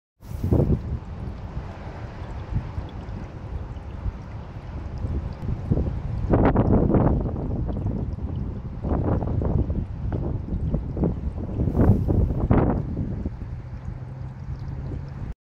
St Ninian's Well
Sounds of a spring in Spring!

Babbling brook by beach!